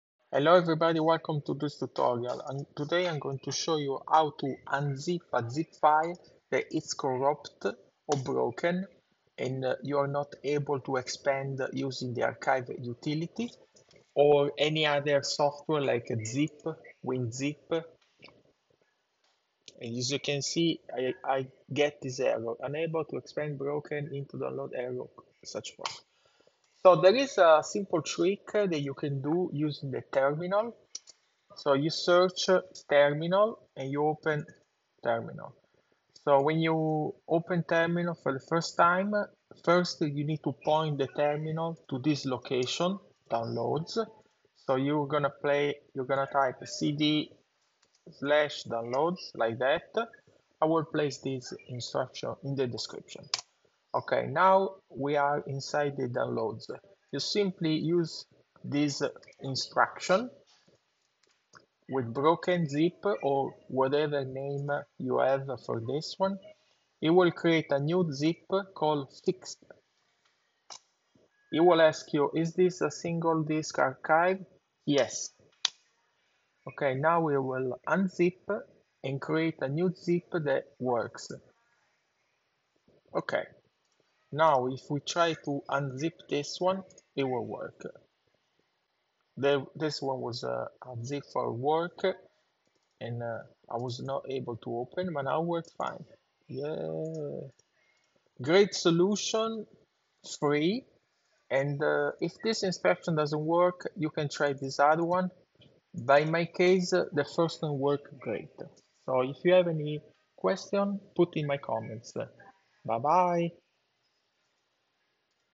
Recover corrupt broken zip file for free with Mac
Open Terminal
type cd ~/Downloads (or folder where the zip is)
zip -FF broken.zip --out fixed.zip
If doesn't work
zip -F broken.zip --out fixed.zip